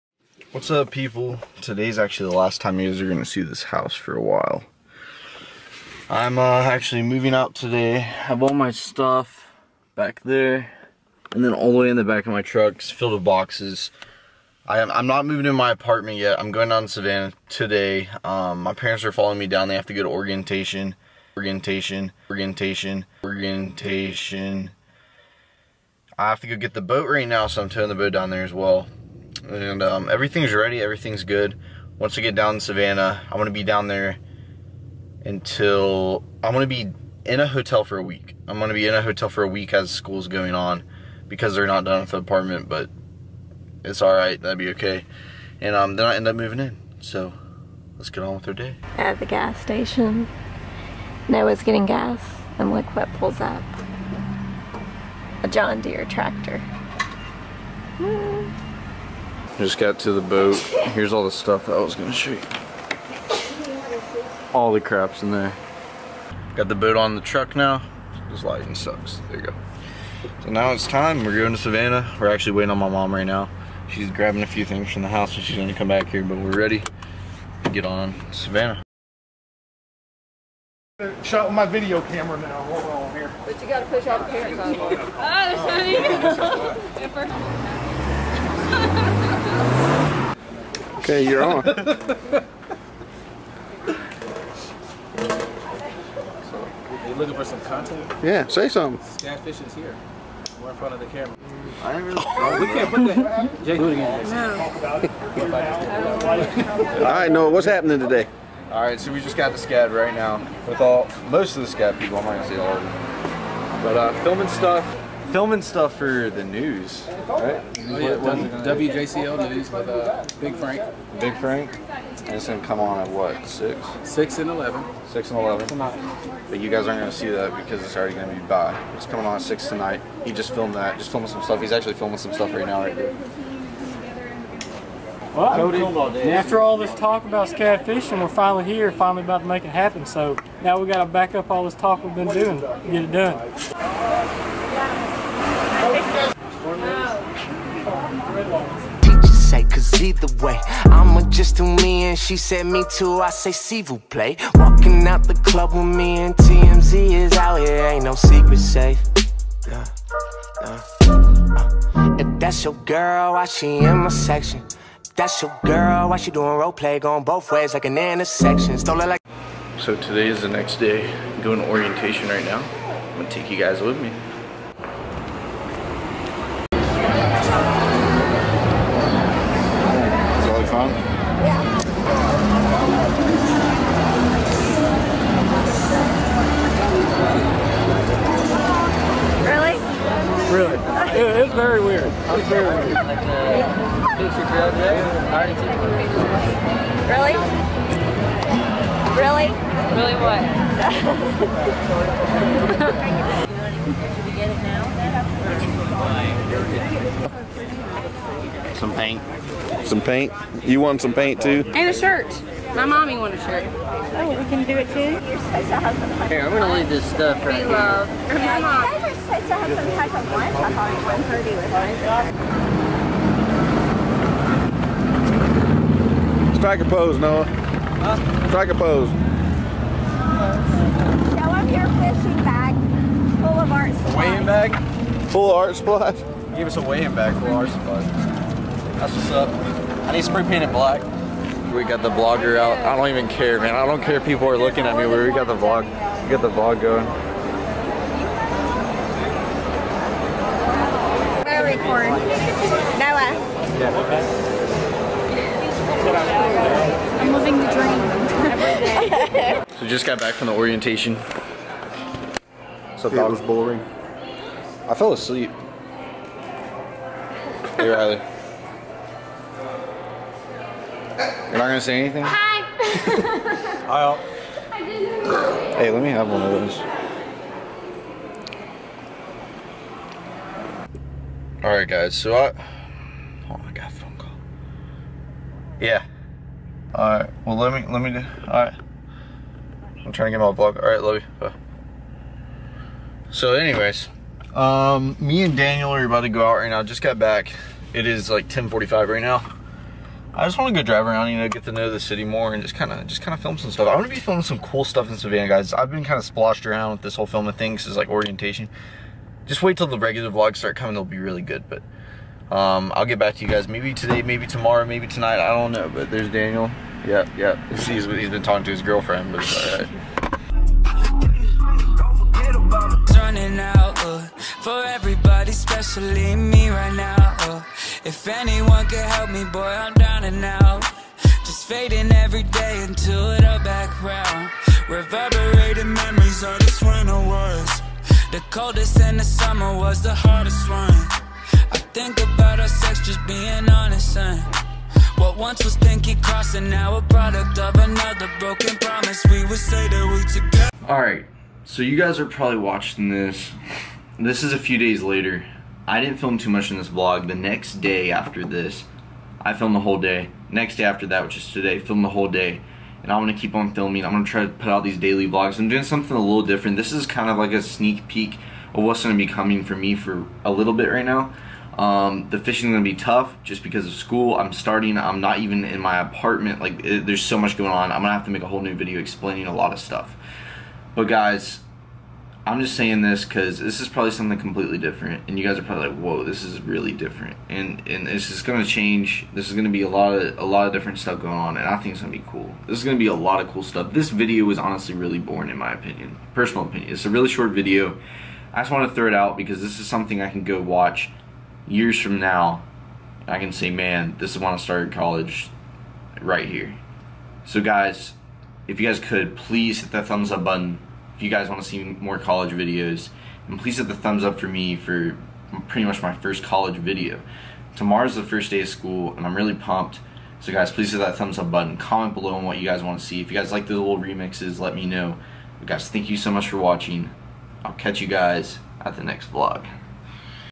I MOVED OUT!
I MOVED OUT! I am moving to Savanah Georgia to start college at SCAD. However, I am not going to be moving into my apartment because there was a delay, so I have to spend the week in a hotel while I am starting classes. It is orientation day and I am going to bring you along with me!